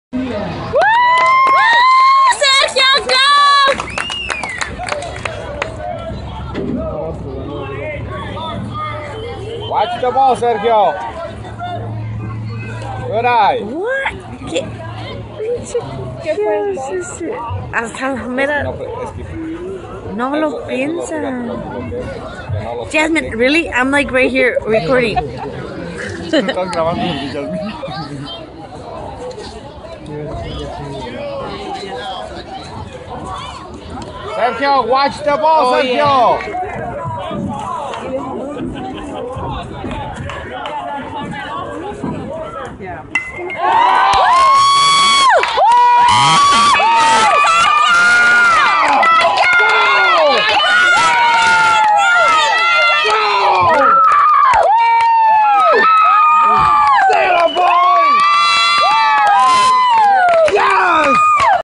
Sergio's 3 RBI Hit at Helfaer Field with Felix Mantilla Little League 6/1/13
9-10 year old Red Team with Felix Mantilla Little League. Milwaukee WI.